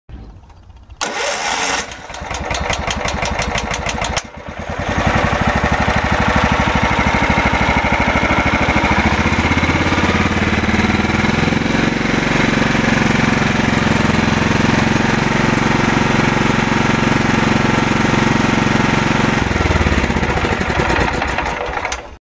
Onlineveilingmeester.nl | Zitmaaier John Deere, LTR155, bouwjaar 2000
Voor alle informatie zie de veilingsite.
Kijkdag(en):
vrijdag 12 april 2019, 13:00 - 16:00 uur
Venus 27 8448CE Heerenveen
Sluiting:
dinsdag 16 april 2019, 19:00 uur